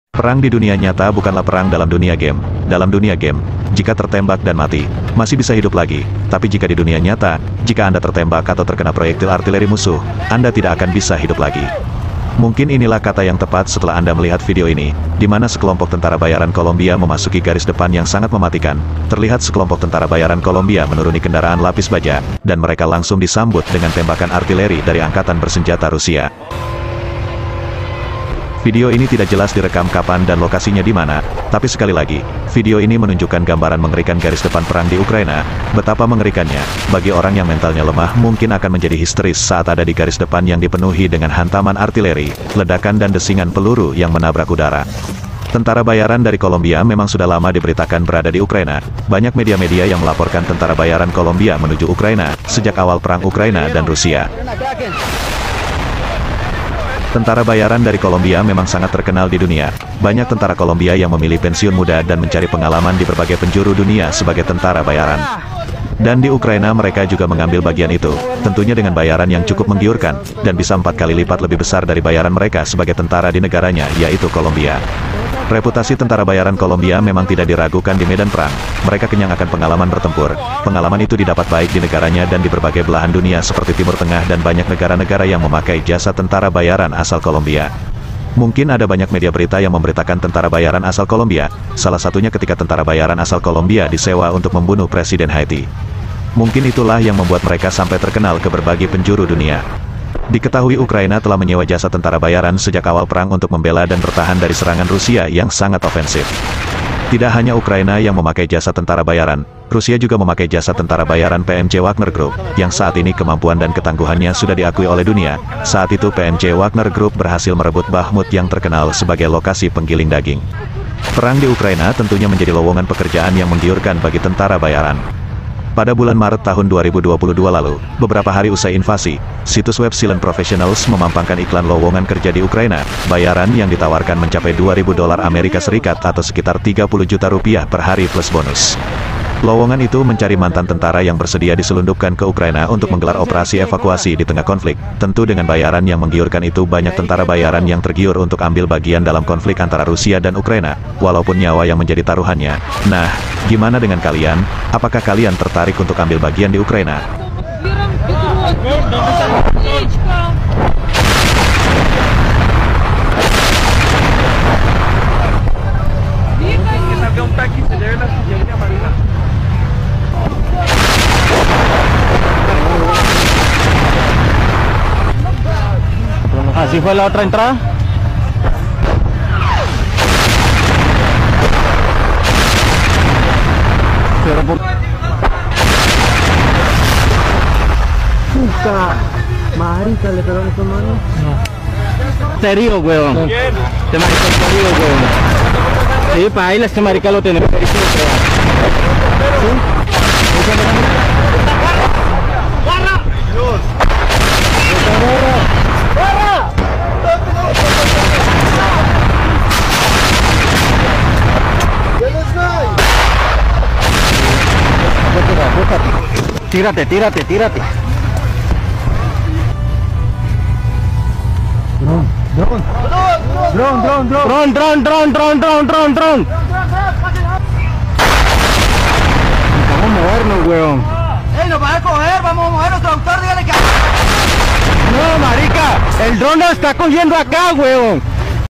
Ngeri!! Tentara Bayaran Kolombia Di Hujani Artileri Rusia
Perang di dunia nyata bukanlah perang dalam dunia game, dalam dunia game, jika tertembak dan mati, masih bisa hidup lagi, tapi jika di dunia nyata, jika anda tertembak atau terkena proyektil artileri musuh, anda tidak akan bisa hidup lagi.
Mungkin inilah kata yang tepat setelah anda melihat video ini, dimana sekelompok tentara bayaran Kolombia memasuki garis depan yang sangat mematikan, terlihat sekelompok Tentara bayaran Kolombia menuruni kendaraan lapis baja, dan mereka langsung di sambut dengan tembakan artileri dari angkatan bersenjata Rusia.